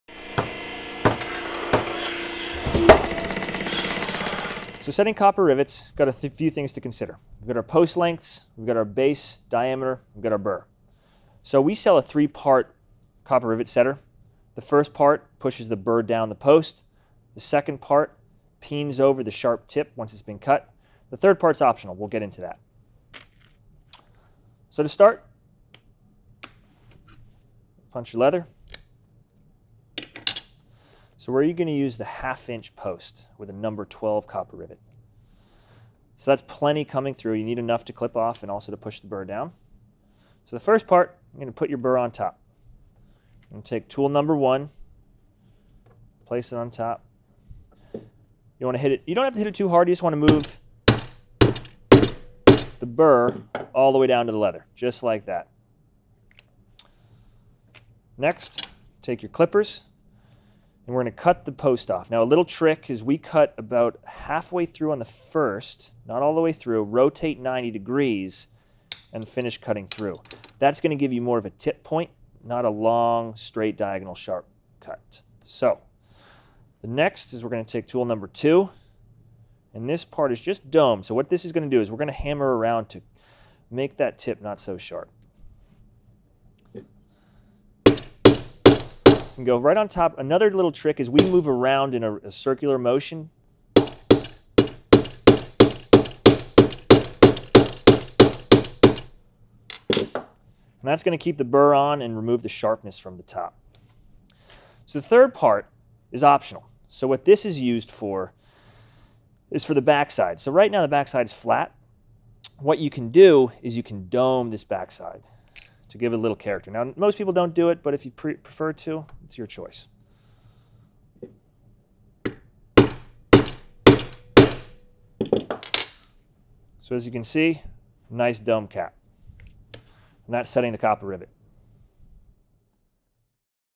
How to Set Copper Rivets in Leather
How To Set Copper Rivets in Leather with Our 3-Part Tool

Our standard line of copper rivets come in sizes #9, #12 and #14. Each of which comes in either a 1/2", 3/4", or 1" post, along with burrs.

We will show you how to use our 3-part tool when setting copper rivets in leather as well as choosing the right size post for your project.

These copper rivet setters are made 200 yards from our front door at a local machine shop and are super easy to use! They also do not leave markings on your leather like the 1-piece copper rivet setters do. As you grow in your leathercraft, we're here to grow with you with upgraded tools that can increase your speed, consistency, and fingers!.

As always, with any further questions feel free to give us a call or email!